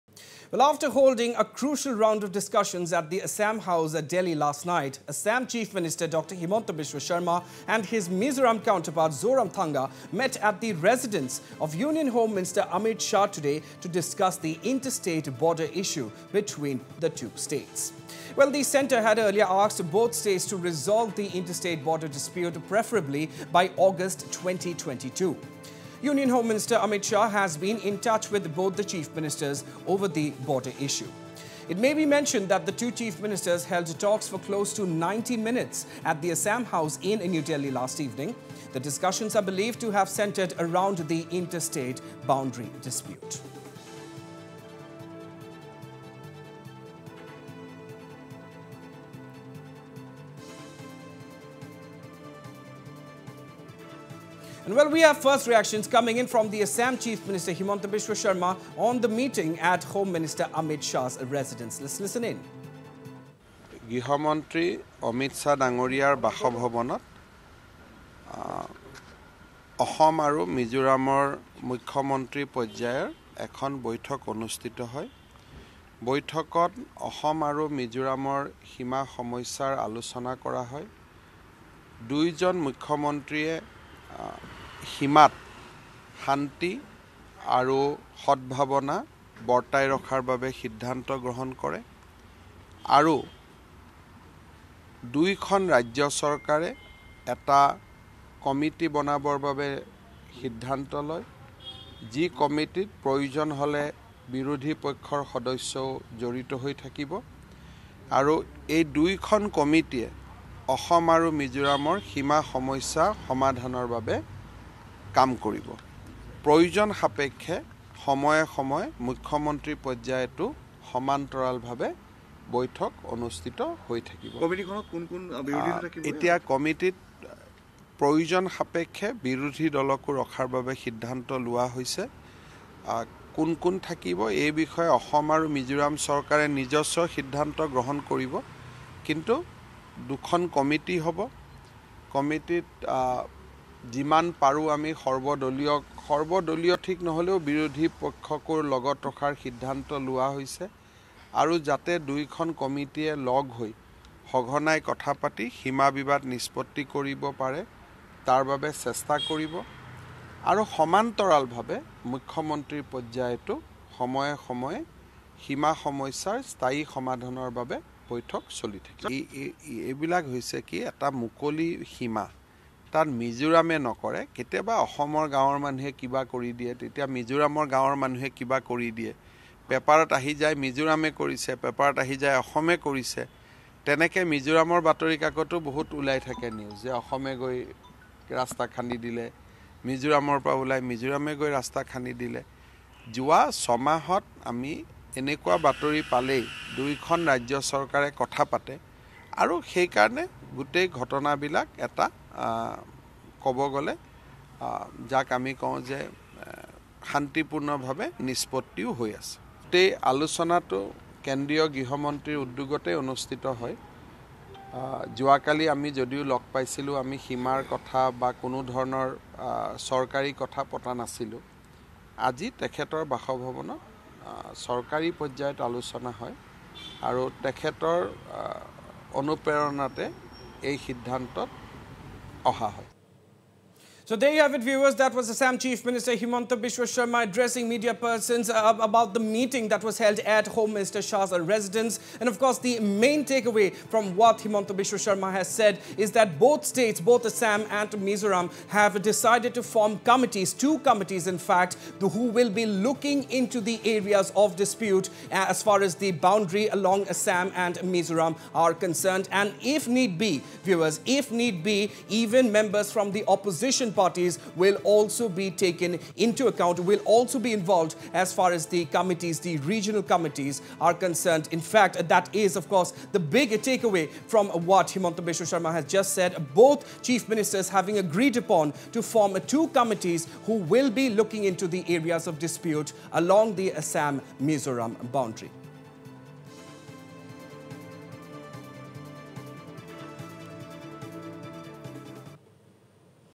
Assam, Mizoram to form two committees to resolve inter-state border dispute
Assam | Mizoram | Assam and Mizoram have decided to form two committees to resolve the inter-state border issue. The decision was taken during a meeting held at the residence of Union Home Minister Amit Shah today.